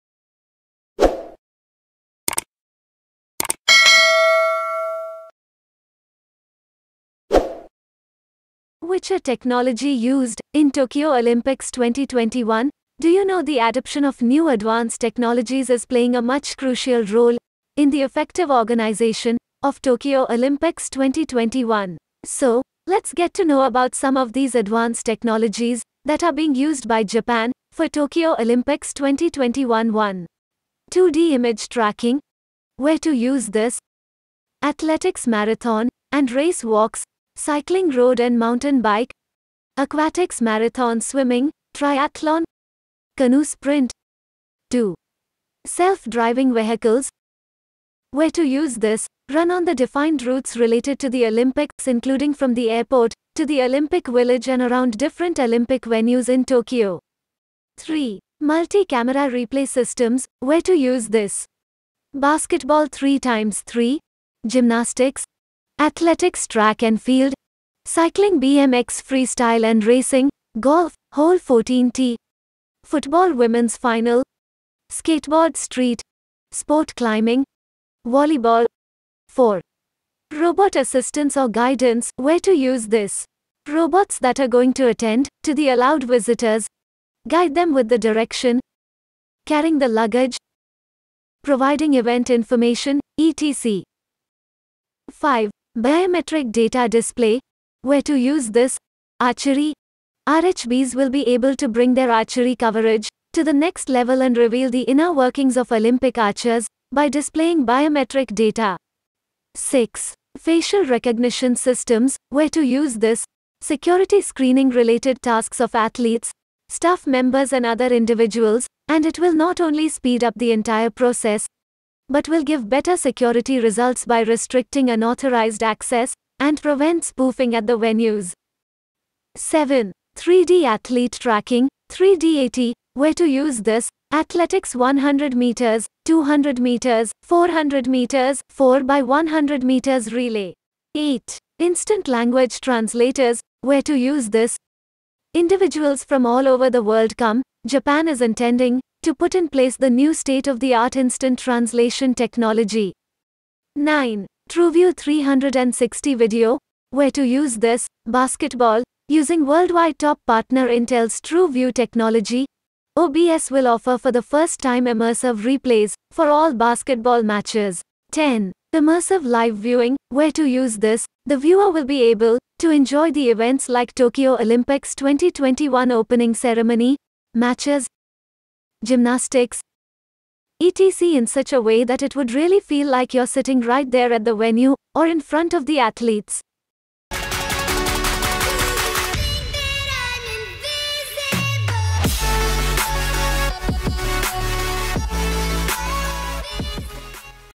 Which are technology used in Tokyo Olympics 2021
Interesting technologies facts which are used in olympics games.